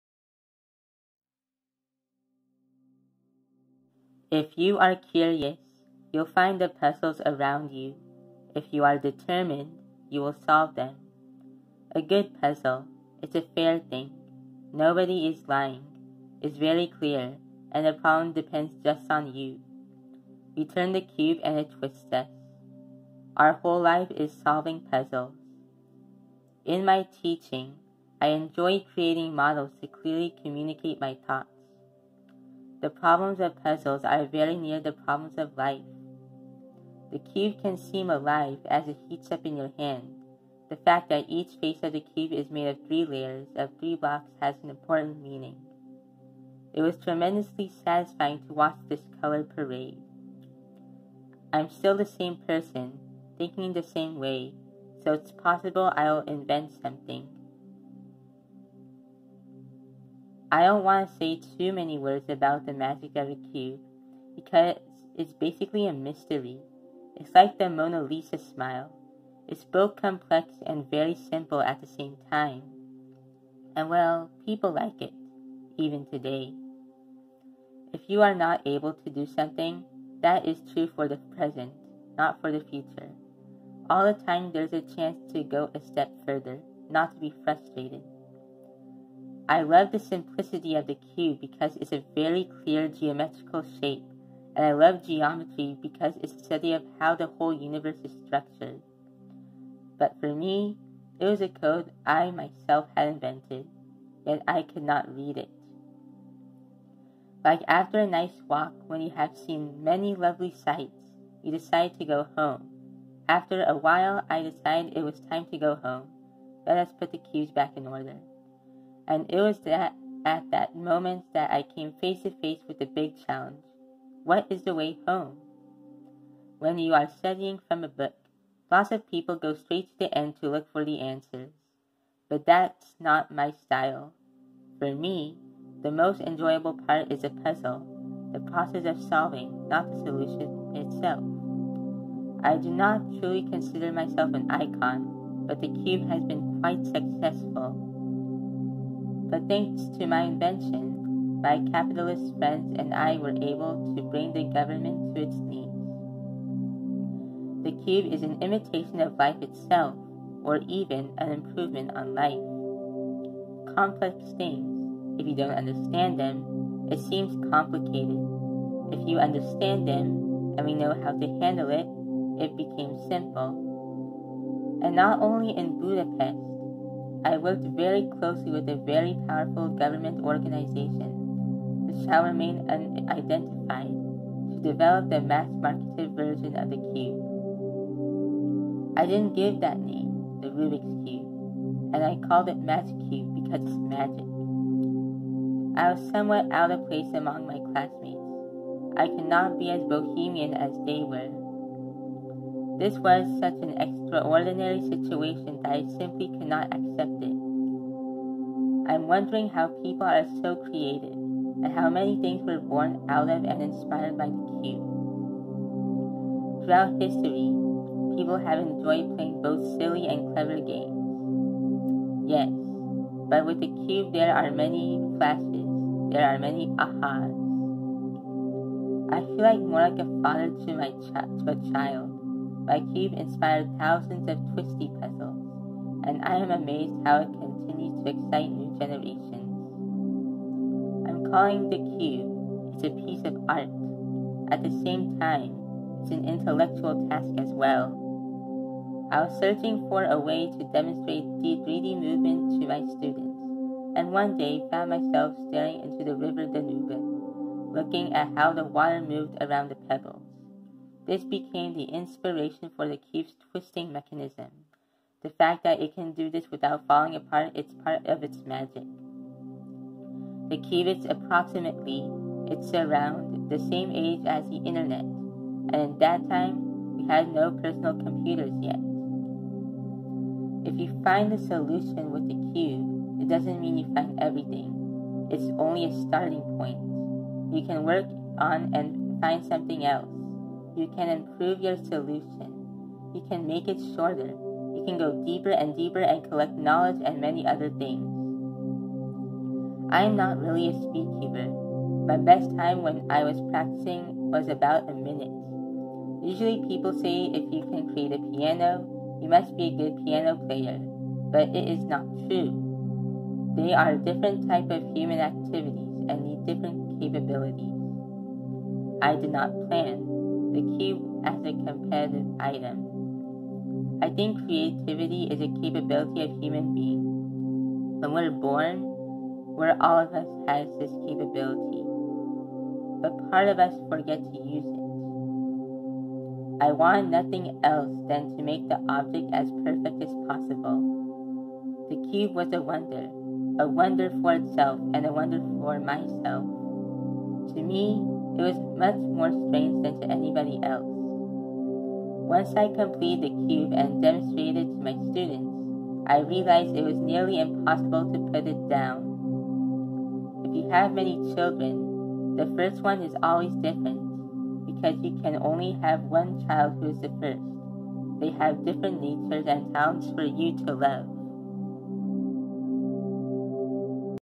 Ernő Rubik Quotes
A compilation of the most inspirational quotes from the inventor - Professor Ernő Rubik.